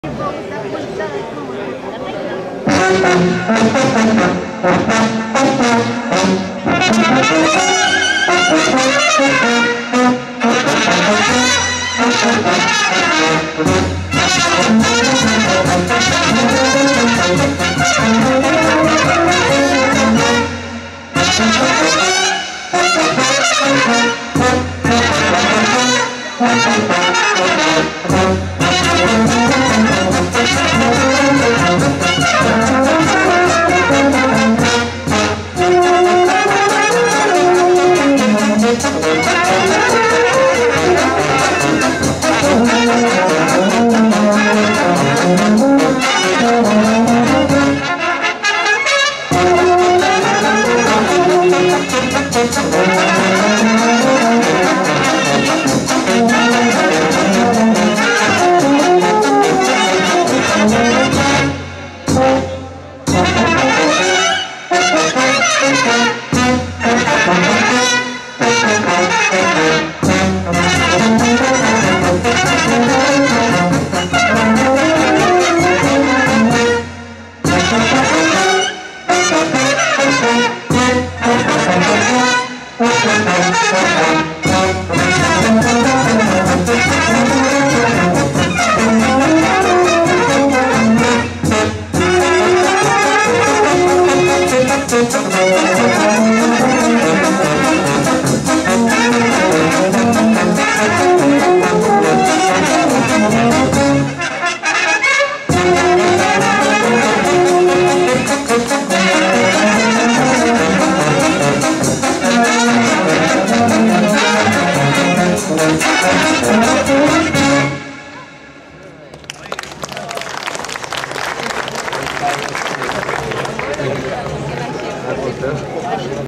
Fanfara Fraţii Lucan - Festivalul Fanfarelor Pădureni 2022 - 📞
Fanfara_Fraţii_Lucan de la Chetriş - Iaşi la Festivalul Fanfarelor Pădureni - 25 septembrie 2022